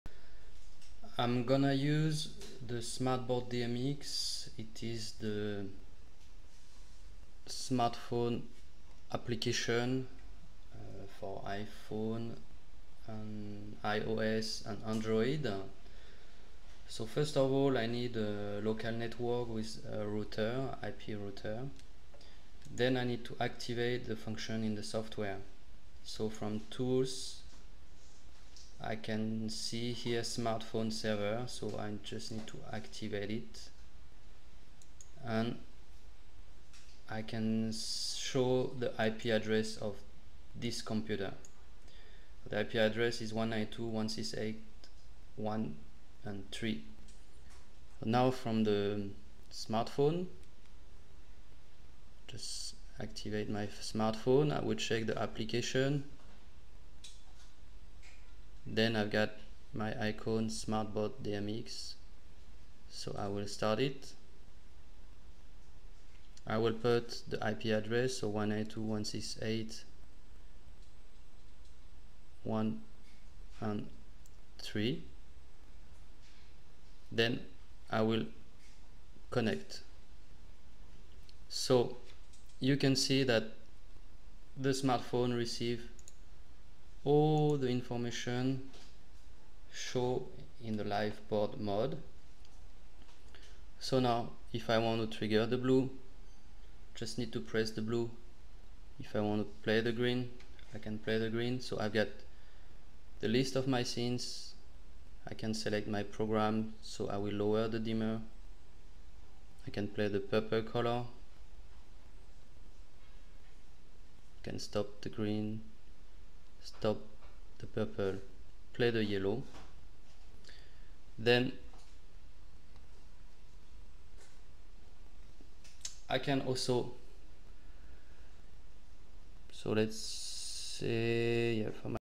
Smartboard DMX demo and tutorial Part 1
Chromateq Smartboard DMX demonstration and tutorial (Part 1). Standard use example of the Free Iphone, Android, Ipad, Ios, tablet, smartphone DMX and DMX512 application (App, Api).